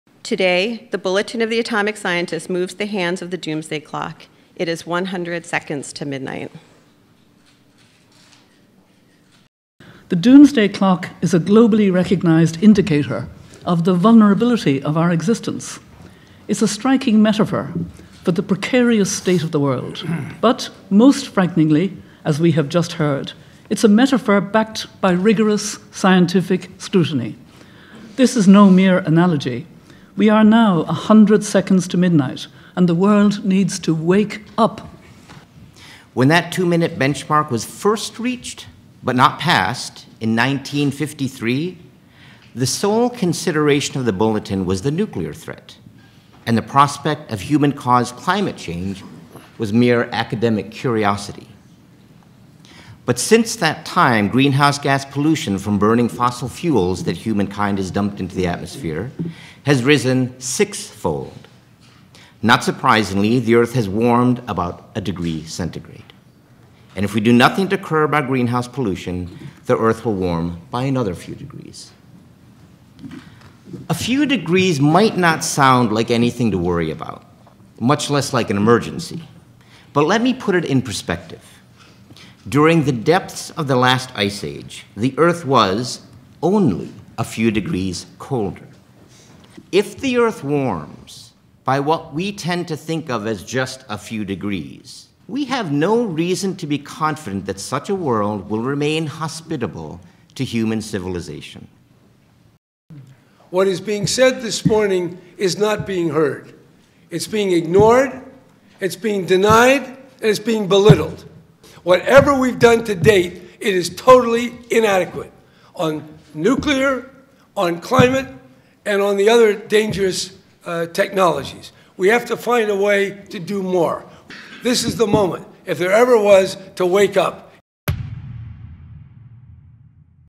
Menaces imminentes de guerre nucléaire et changement climatique - L'Horloge de l'Apocalypse
Depuis le 23 janvier 2020, l'horloge de la fin du monde affiche minuit moins cent secondes en raison de « l'incapacité des dirigeants mondiaux à faire face aux menaces imminentes d'une guerre nucléaire et du changement climatique ».

Le Bulletin of the Atomic Scientists est une revue scientifique non technique, en ligne, qui traite de la sécurité mondiale et les questions de politique publique, en particulier celles liées aux dangers posés par les armes nucléaires et autres armes de destruction massive. (Wikipédia)